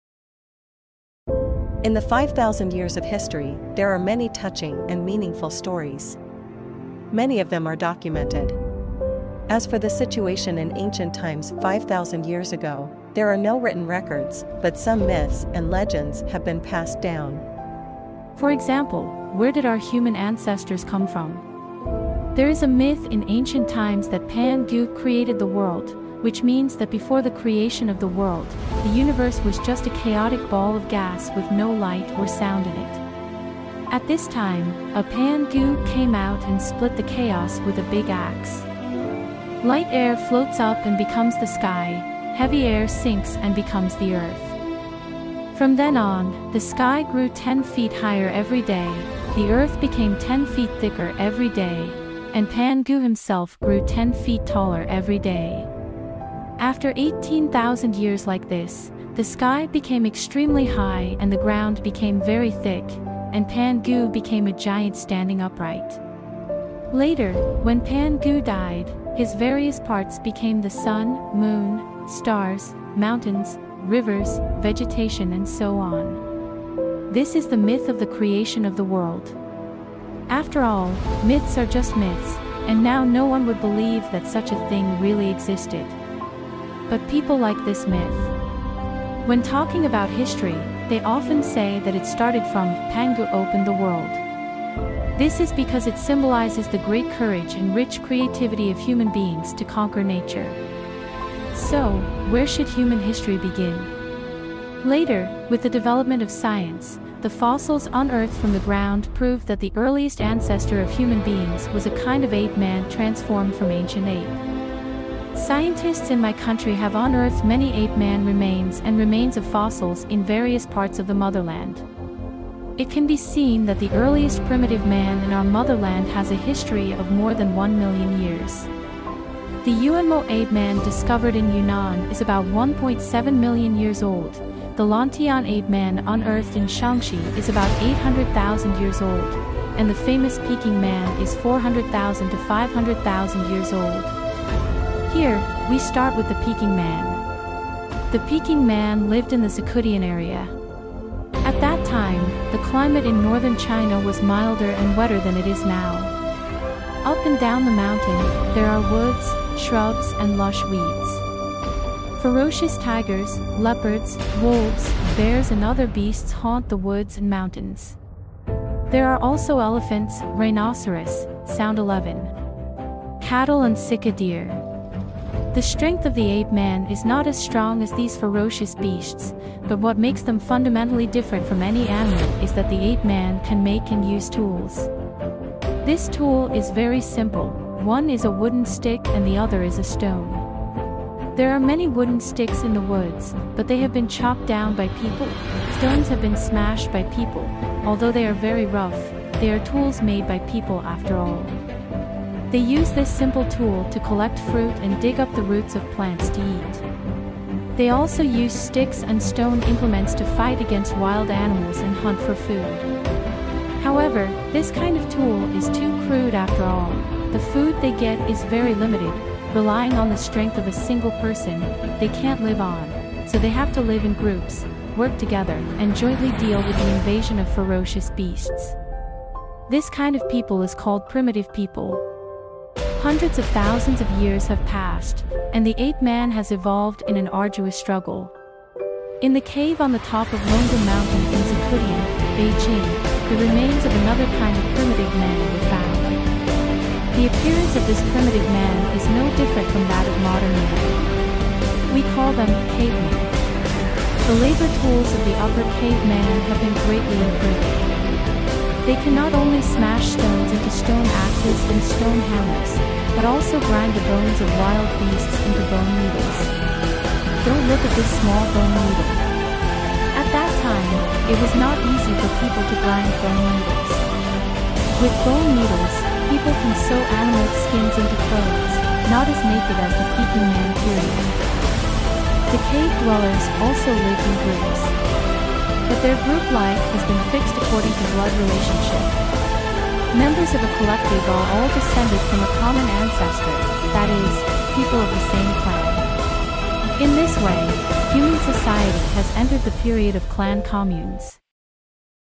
Chinese history Five Thousand Years of China（上下五千年）
This is a video about how the earliest China was formed, explaining every bit of China from its formation to the present from a historical perspective